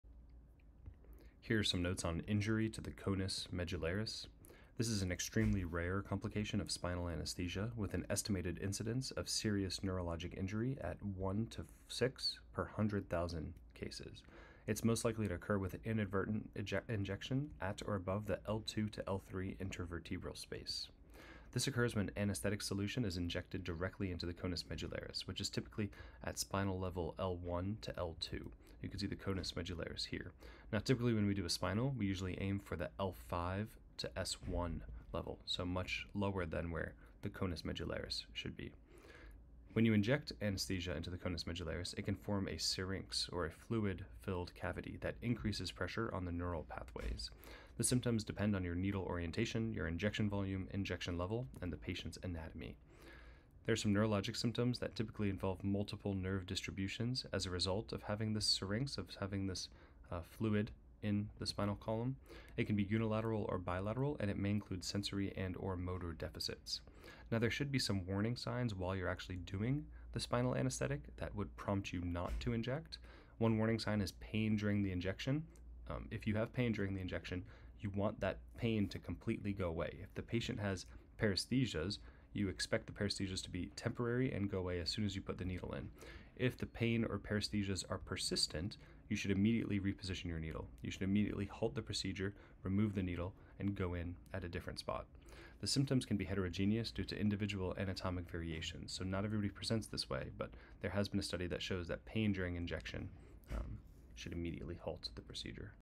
Injury to the conus medullaris
Rarity and Incidence:
- Extremely rare complication of spinal anesthesia
- Estimated incidence of serious neurologic injury is 1-6 per 100,000 cases
- Most likely to occur with inadvertent injection at or above L2-L3 intervertebral space

Mechanism:
- Occurs when anesthetic solution is injected directly into the conus medullaris (typically ending at L1-L2)
- Can form a syrinx (fluid-filled cavity) that increases pressure on neural pathways
- Symptoms depend on needle orientation, injection volume, injection level, and patient anatomy

Clinical Characteristics:
- Neurologic symptoms typically involve multiple nerve distributions
- Can be unilateral or bilateral
- May include sensory and/or motor deficits

Warning Signs:
- Pain during injection should immediately halt the procedure
- Persistent paresthesia requires needle repositioning
- Symptoms can be heterogeneous due to individual anatomical variations